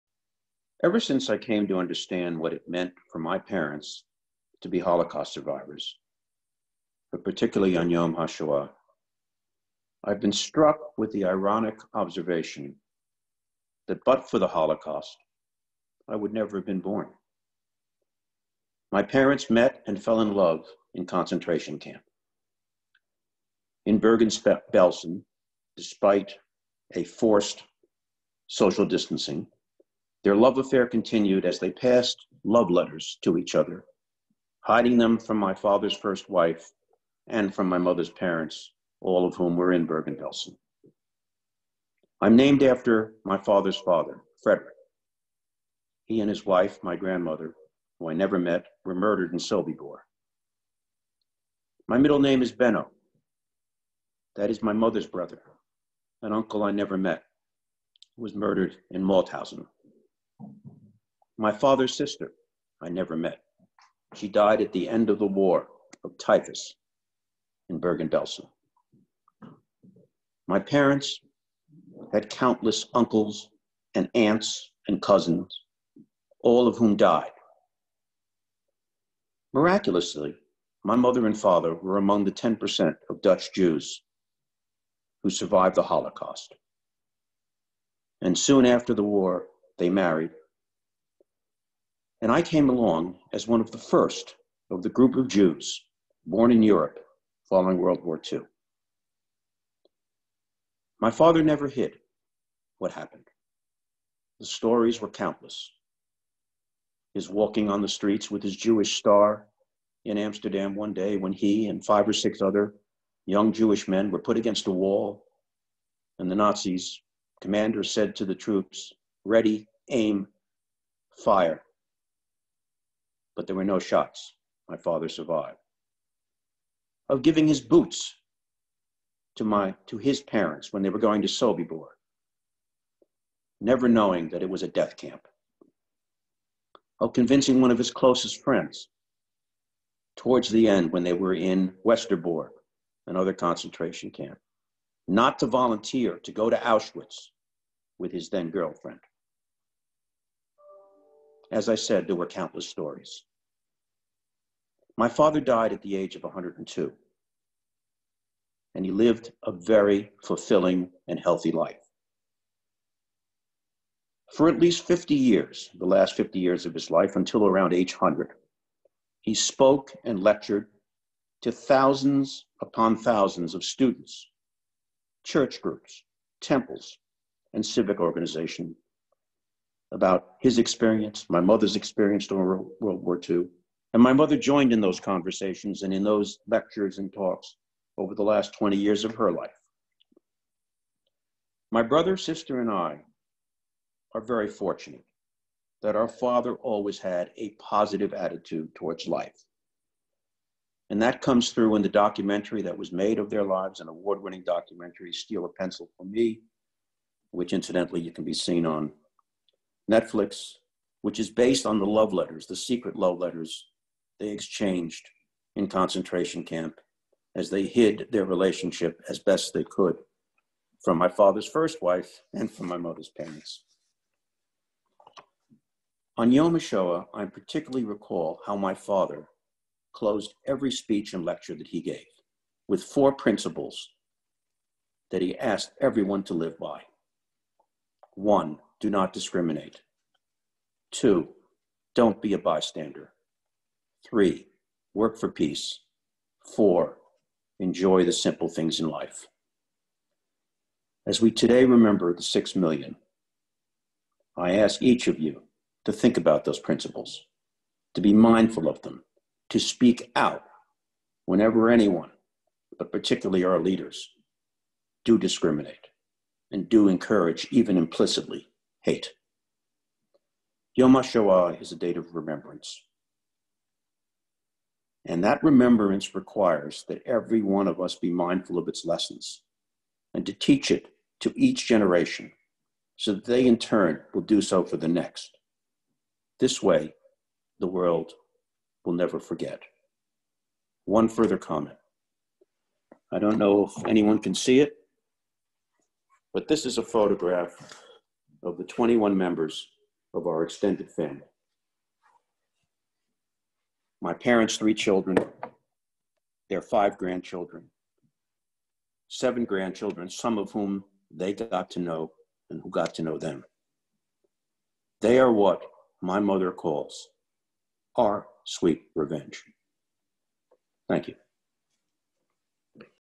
The story of survival during the Holocaust - Fred Polak
Without the Holocaust, Fred would have never been born. His parents met and fell in love in a concentration camp.